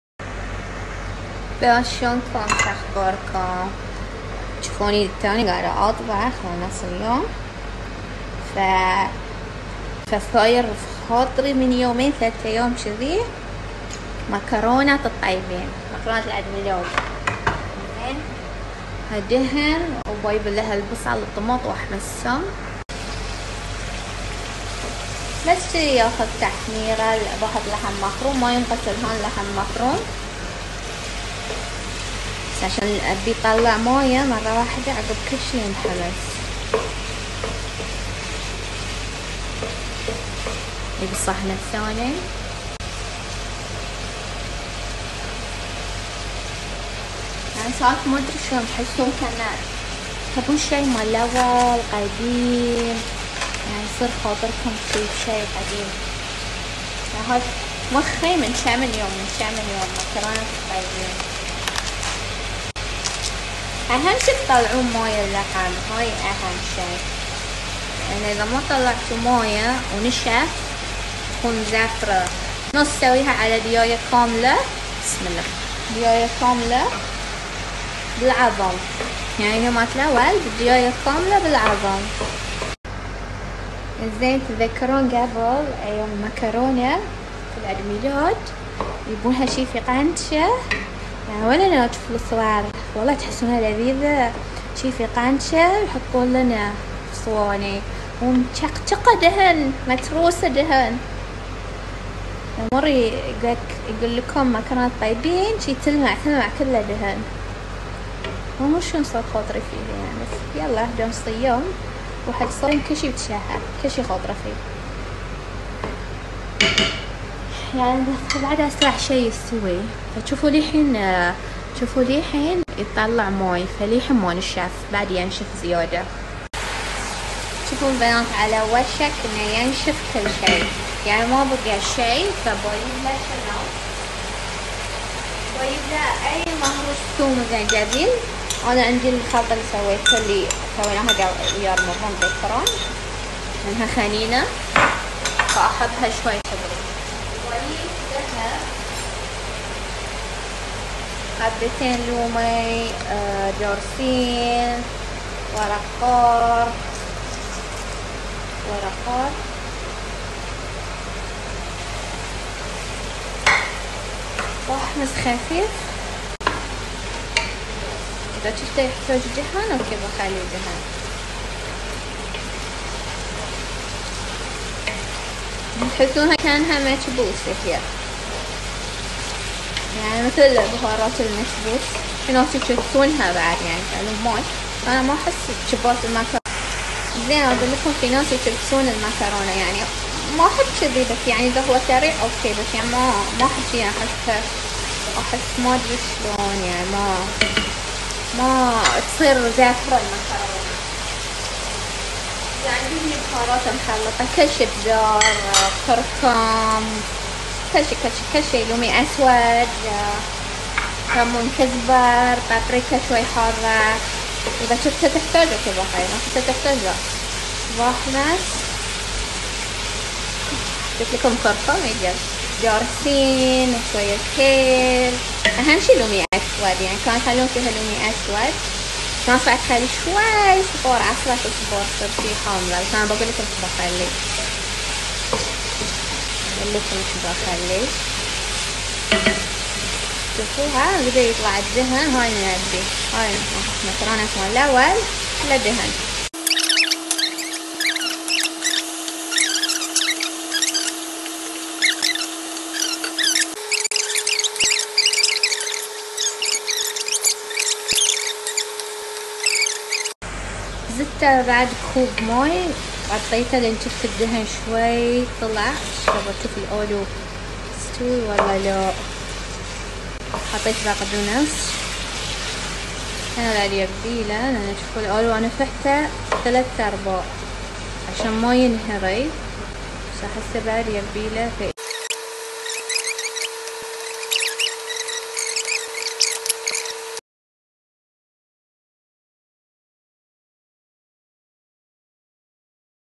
معكرونة الطيبين بطريقة عمانية مرة حلوة
المقادير
کوبين ماي سلق المعکرونه ضروري للنكهة
لحم مفروم
خضار فروزن
بطاطس مسلوقه ومقطعه
بصل وطماط مقطع
بهارات صحيحه ليمون اسود عود دارسین ورق غار
بهارات مشکله: بزار بحريني ودارسين ولیمون اسود بودر وهیل و کركم وببريكا وماجي وملح وفلفل اسود وبقدونس مفروم للزینه
اضافه اجار ليمون من مخل لات الجزیره